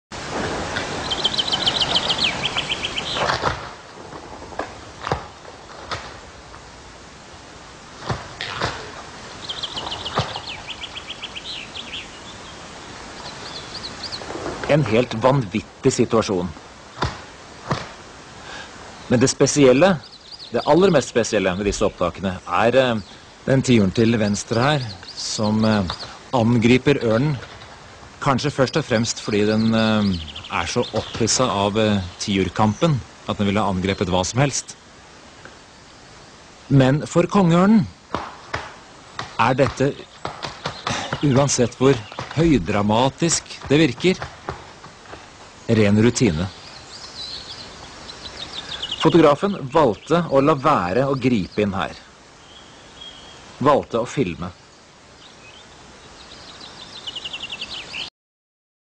Nature Is Awesome Golden eagle(King Eagle) takes down two Wood grouses
Golden eagle(King Eagle) takes down two Wood grouses